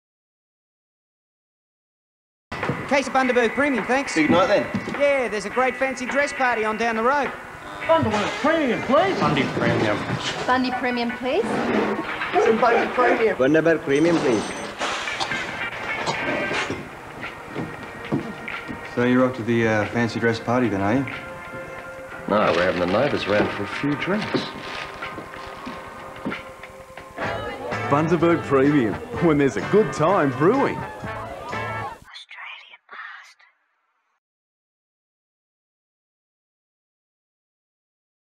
Bundaberg Premium Party Rum Infused Brewed Beverage 1990s Advertisement Australia Commercial Ad
Swingers & Costume Parties Bundaberg Premium Party Rum Infused Brewed Beverage 1990s Advertisement Australia Commercial Ad